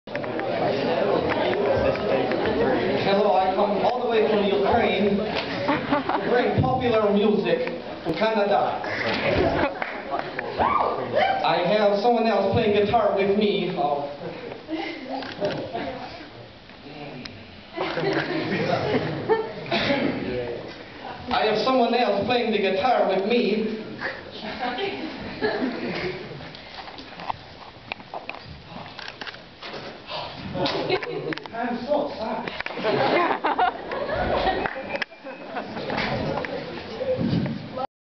Ukrainian Entrance
The brief skit before the two perform some Metro Station and some Katy Perry at this year's Coffee House.